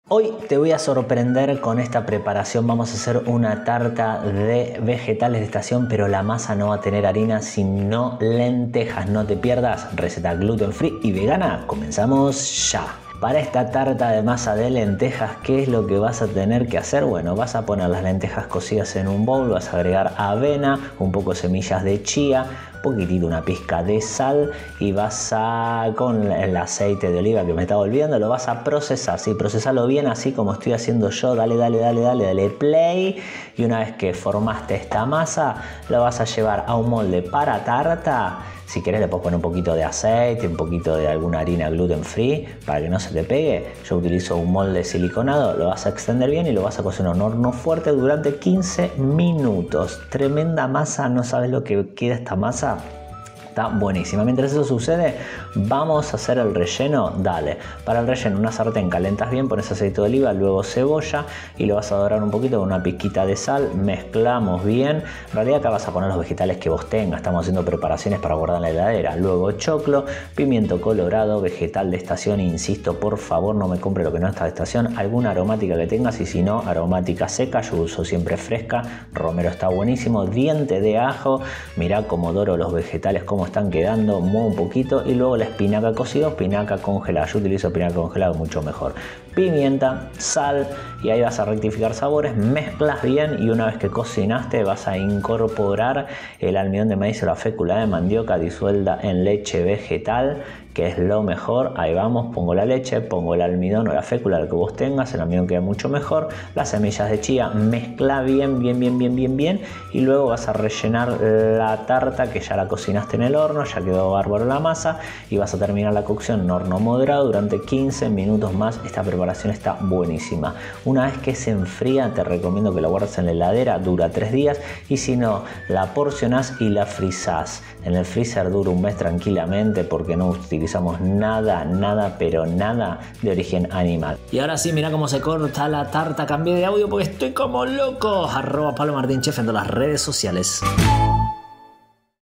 🍃 TARTA CON MASA DE LENTEJAS 100% VEGANA Y GLUTEN FREE 🍃
A pedido de todos ustedes, hoy les regalo esta tarta con masa de lentejas. Sí, leíste bien. La masa la hice con lentejas y sin harina de trigo. Es una bomba 💣💥
Además utilicé semillas de chía, por sus propiedades nutricionales y por sus mucílagos.
Acá te detallo los ingredientes; el paso a paso de la receta, lo podrás ver en el video.

INGREDIENTES
🔹Para la masa de lentejas:
✔️ 2 tazas de lentejas cocidas
✔️ 3 cucharadas de avena tradicional (para que sea gluten free tiene que estar certificada)
✔️ 1 cdita. de sal marina
✔️ 1 cda. de semillas de chía
🔹Para el relleno:
✔️ 1 cebolla
✔️ 1 morrón rojo
✔️ 1 choclo
✔️ 1 ramita de romero
✔️ 2 paquetes de espinacas o 3 tazas de espinacas congeladas
✔️ 1 taza de leche vegetal
✔️ 2 cditas. de almidón de maíz
✔️ Sal marina, pimienta y aceite de oliva, C/N

 ✍Tips para la receta: ✍
✍ Cociná la base de la tarta, en un horno moderado, durante 15 minutos.
✍ Luego, cociná la tarta durante 15 minutos más.
✍ ¡Compartí la receta, porque es tremendamente rica!

😃 Contame qué te pareció la receta.
😃 ¿Qué relleno hacés?
😃 ¿Alguna vez hiciste una masa así?

🤓 ¡Te leo acá en los comentarios!